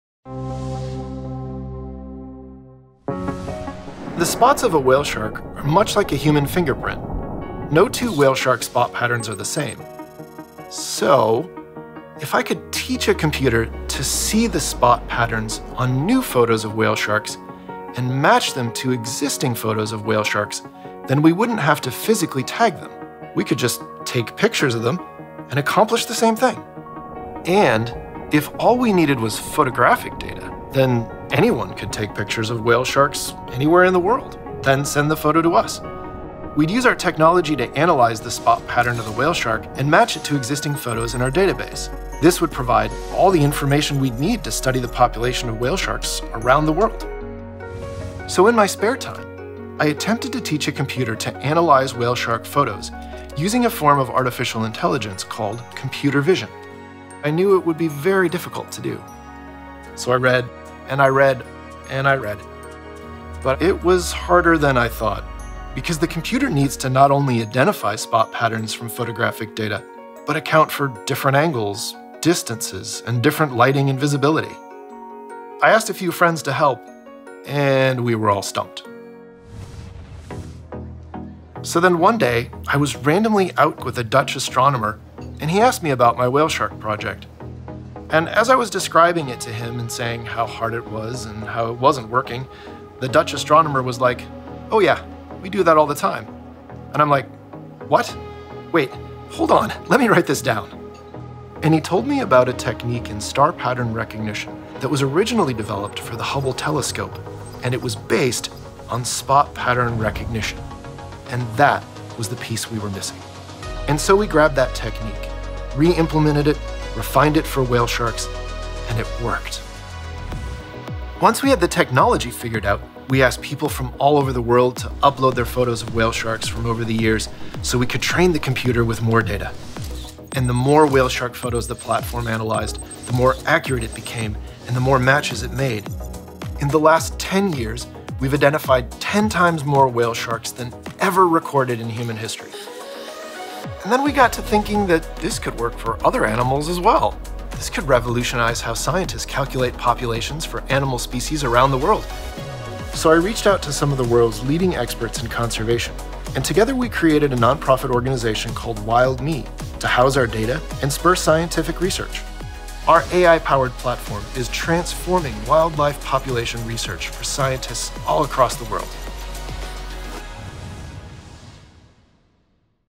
Wild Me: The Solution
Tech for Global Good 2021-22
Laureate: Wild Me

Wild Me’s technology blends structured wildlife research with artificial intelligence, community input and computer vision to speed population analysis of animal species around the globe and develop new insights to bring an end to extinction, through the contributions of hobbyists and scientists.

Music Licensing Sync ID MB01RTTRS6OR0D0